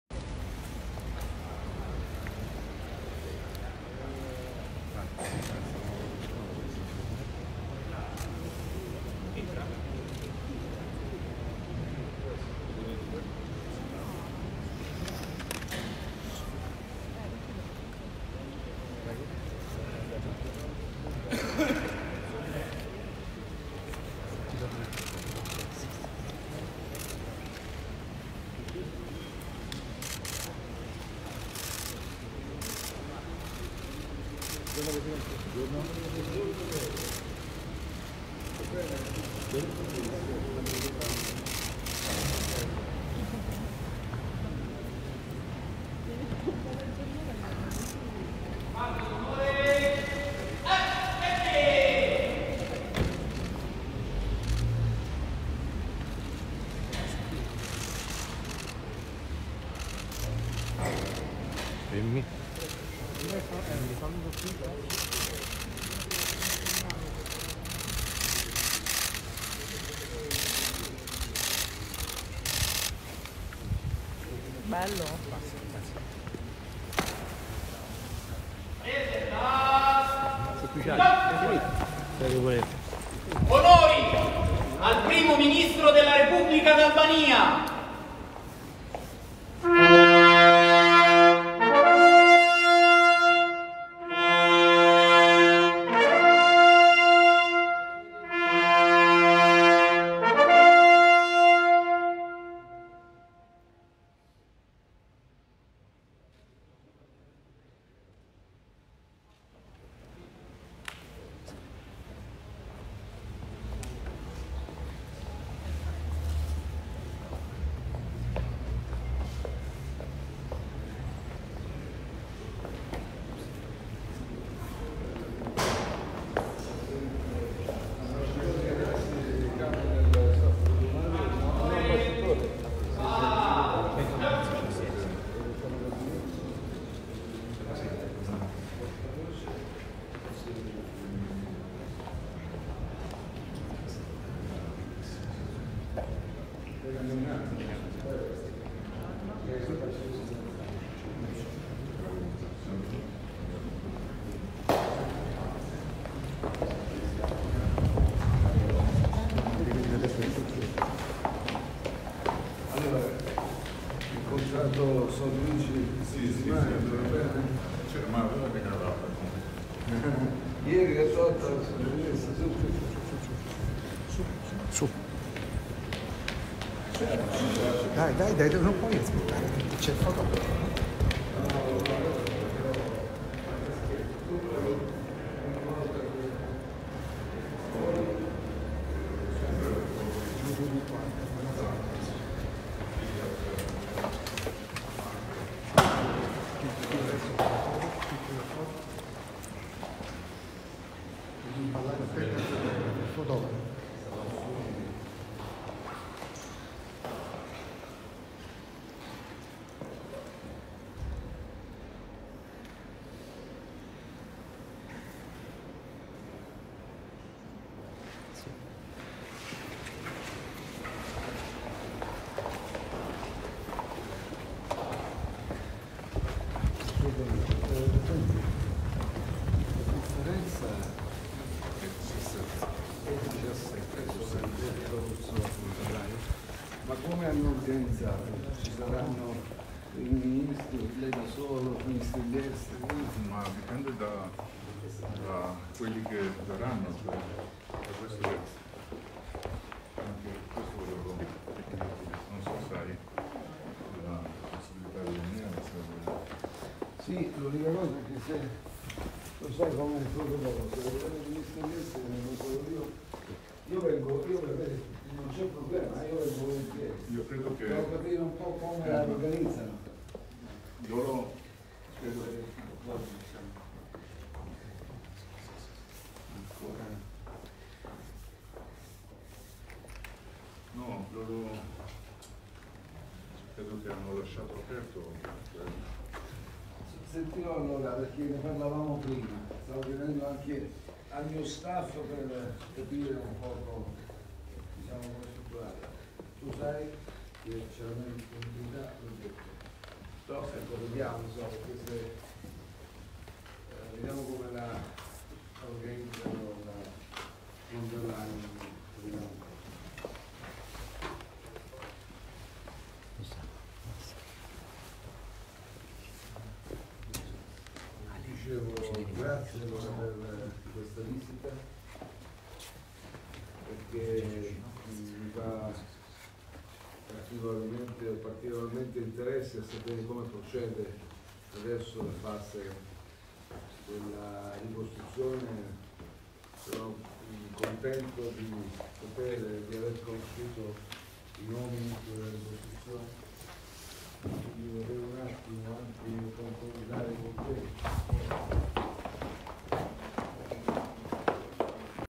Report TV - Rama pritet me ceremoni shtetërore nga Conte
Pas takimit me ministrin e Jashtëm Di Maio, kryeministri Edi Rama është pritur me ceremoni shtetërore nga homologu i tij italian Giuseppe Conte. Rama thotë se takimi po zhvillohet për mbështetjen e mëtejshme të Italisë mike në funksion të Rindërtimit, në vigjilje të Konferencës së Donatorëve më 17 shkurt në Bruksel.